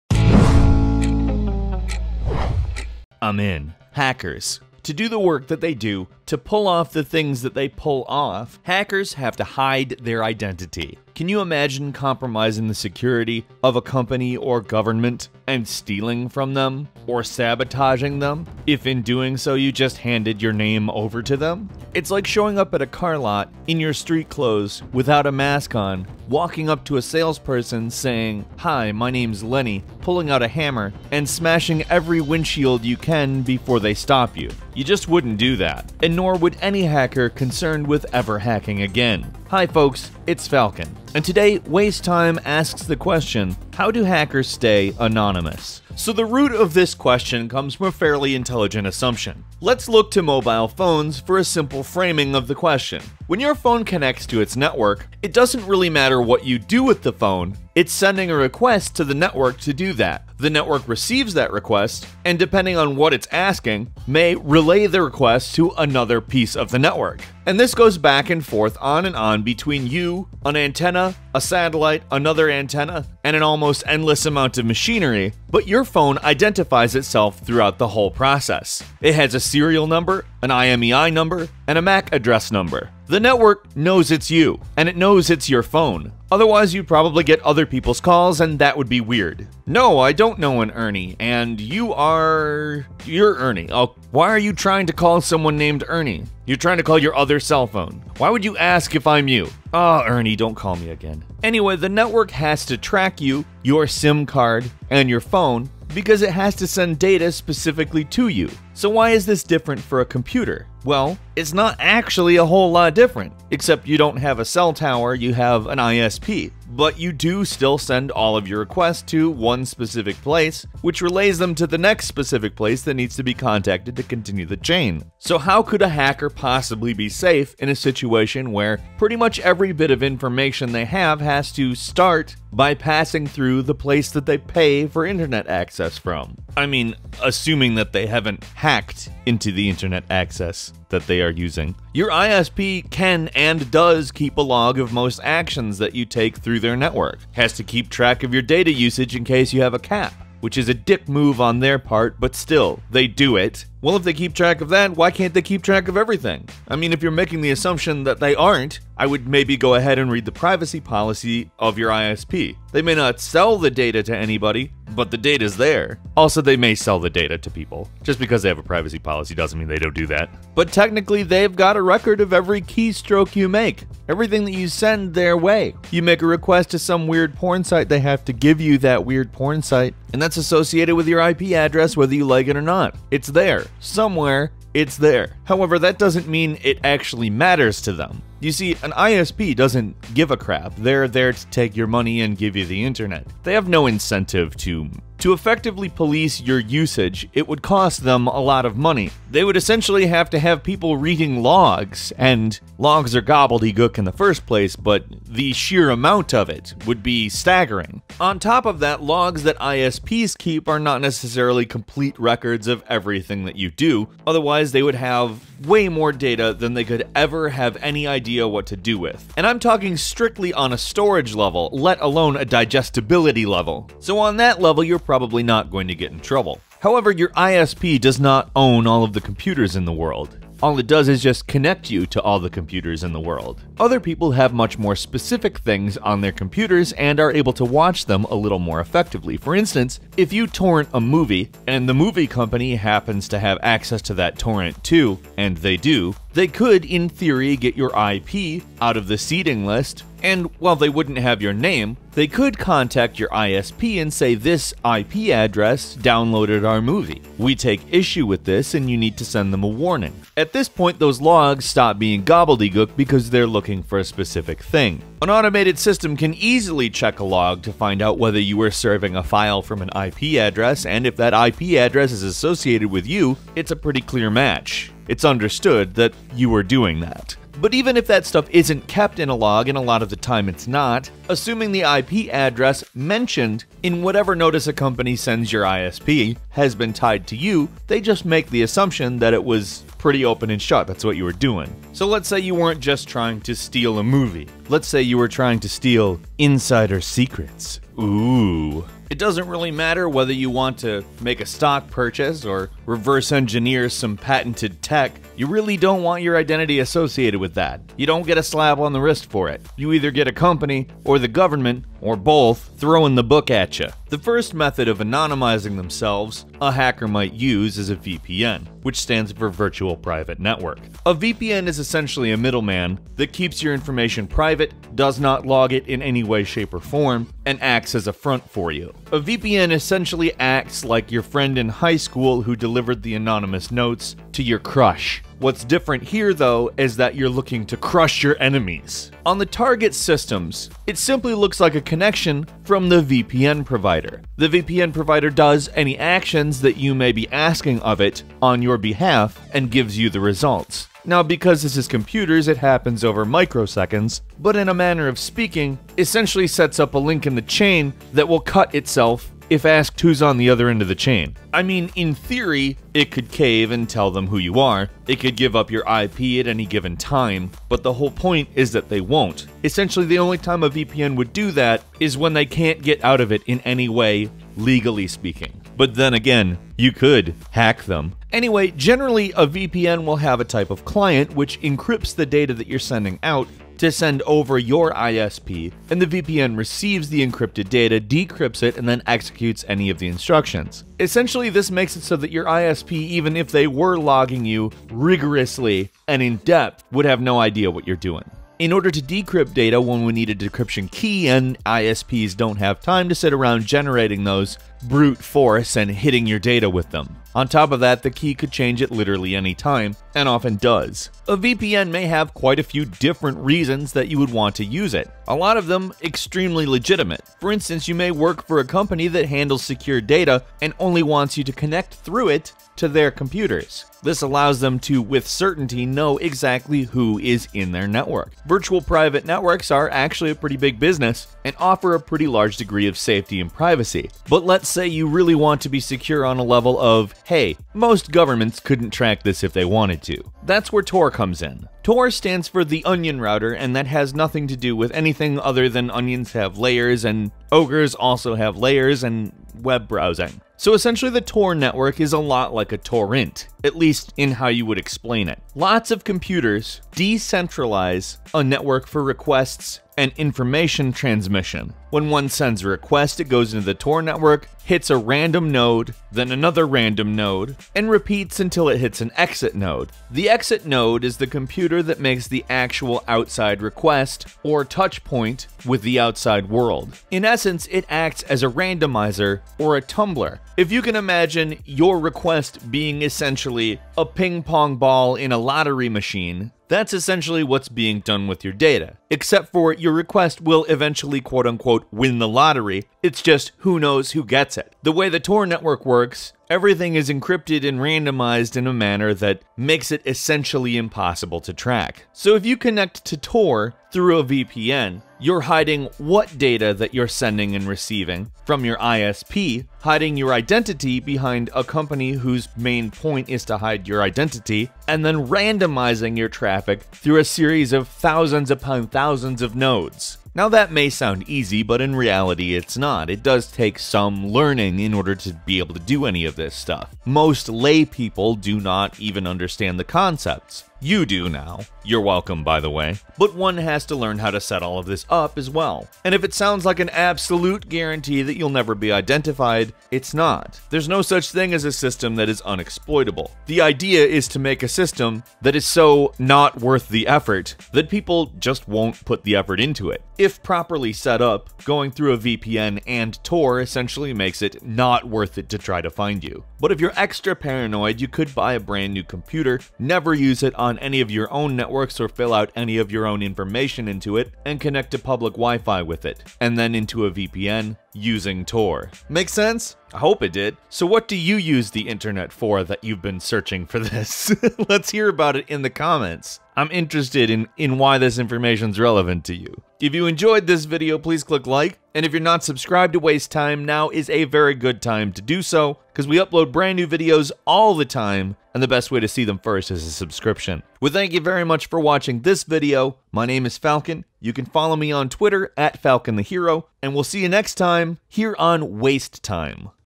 How Do Hackers Stay ANONYMOUS?
Hacking is illegal, yet many people do it. How do they manage not to give away who they are and get into big trouble? Here's the basics!